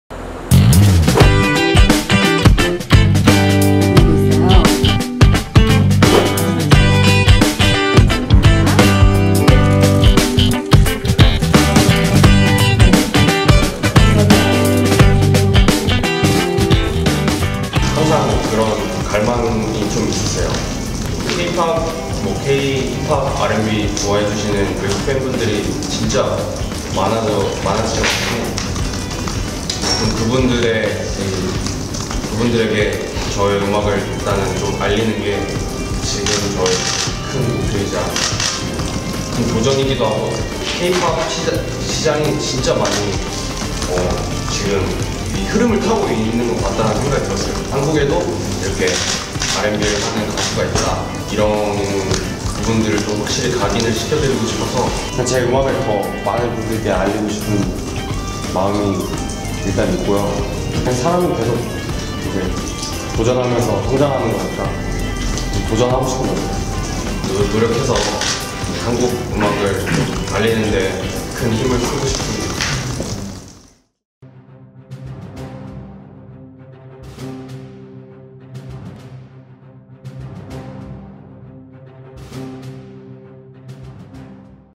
[기자간담회] 크러쉬 “해외 진출 문 꾸준히 두드릴 것”
케이팝의 세계화!
케이팝을 널리 알릴 뮤지션들을 만나고 왔습니다.
all 바른 경제 매체
시사저널이코노미